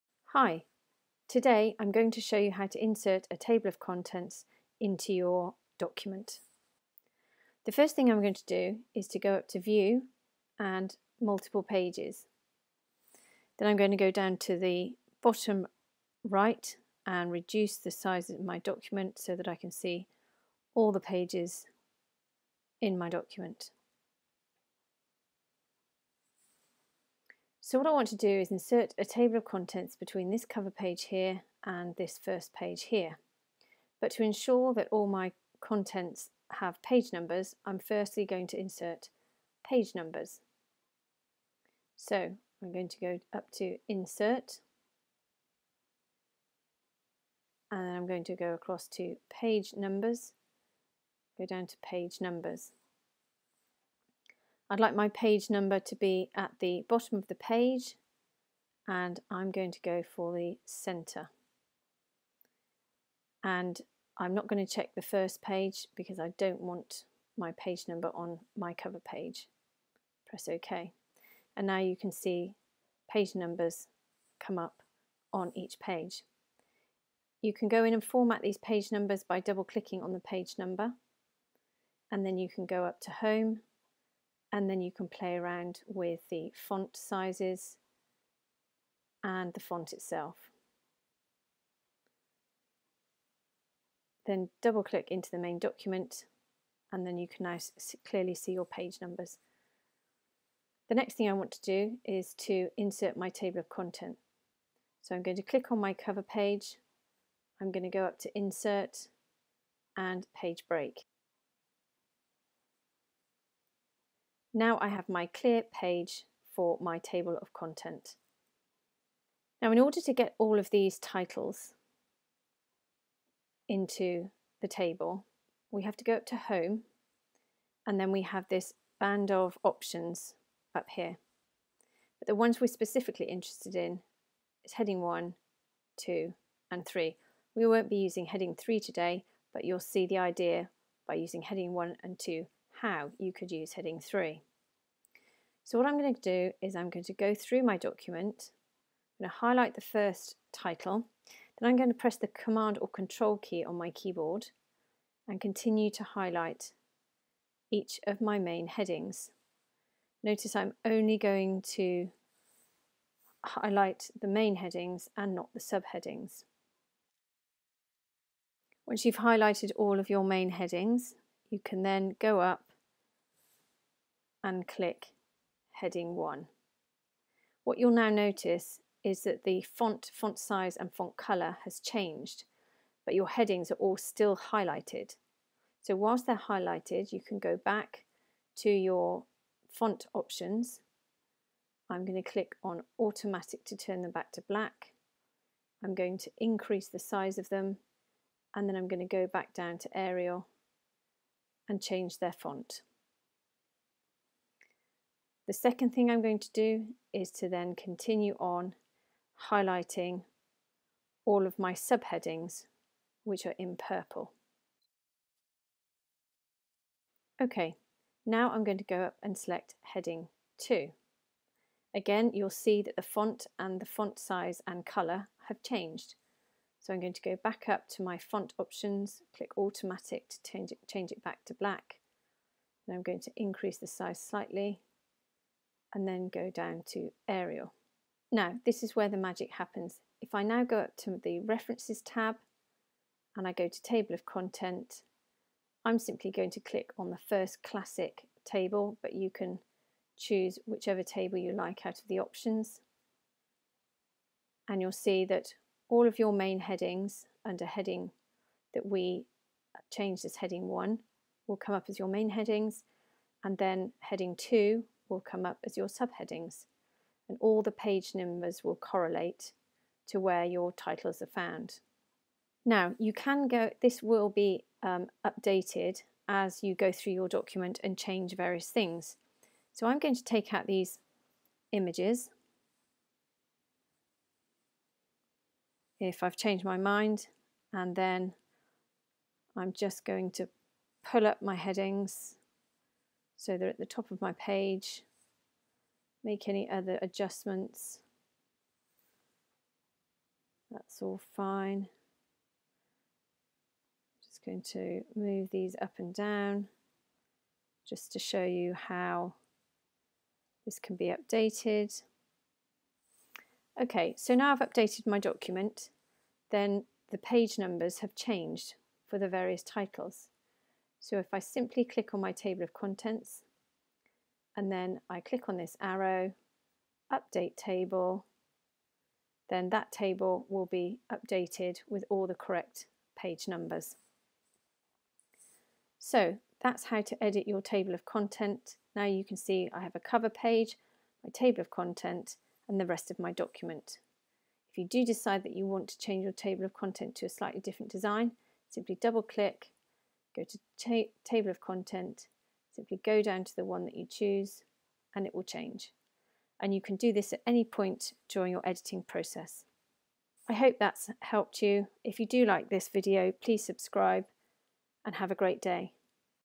How to Create a Table of Contents in Word | Microsoft Word Tutorial
In this step-by-step 'How to Create a Table of Contents in Word' tutorial I show you the simplest way to set up a perfect table of contents that's automated when editing your work. So you've come to the end of your document, story, report, thesis, etc and now you need to complete all those detail bits to formulate your project into a professional looking document. This, of course, includes a TABLE OF CONTENTS, allowing your reader to have a brief overview of what is contained in the document but also to allow them to turn to the page they are most interested in reading. Don't know how to do it. LOOK NO FURTHER!! This QUICK and SIMPLE guide helps you to navigate the Word toolbars to achieve a smart and professional looking table of contents.

This video show you how to insert a Table of Contents within a document.  I show you how to insert a blank page between the cover page and first page of content and then how to select the titles for the table.  I also show you how to select subheadings for the table and how to update the table as you edit your document. I also show you how to insert page numbers, edit them and change the font.  Further more I show you how to view multiple pages of your document in order to make the process easier and quicker.

A list of titles of the parts of a book or document, organized in the order in which the parts appear.

Table of contents can be used in a thesis, dissertation, book, ebook, instruction manual, training manual, education manual, training pack for a workshop, a report, an exam paper, an essay or a paper.  The table of content will help guide your reader through the document or to a particular section they wish to read.

You cannot generate any automatic tables without first using styles effectively throughout your document.  Microsoft Word can scan your document and find everything in the Heading 1 style and put that on the first level of your table of contents, put any Heading 2’s on the second level of your table of contents, and so on.

If you want an automatic table of contents you need to label all of your chapter titles and front matter headings (e.g. “Dedication” and “Acknowledgements”) in the style Heading 1.  All major headings within your chapters should be labeled Heading 2.  All subheadings should be labeled Heading 3, and so on.

If you have used Heading styles in your document, creating an automatic table of contents is easy.

Place your cursor where you want your table of contents to be.
On the References Ribbon, in the Table of Contents Group, click on the arrow next to the Table of Contents icon, and select Insert Table of Contents….

If you want to change which headings appear in your table of contents, you can do so by changing the number in the Show levels: pulldown.
Click OK to insert your table of contents.

The table of contents is a snapshot of the headings and page numbers in your document. At any time, you can update it by right-clicking on it and selecting Update field.  Notice that once the table of contents is in your document, it will turn gray if you click on it. This indicates that it is getting information from somewhere else.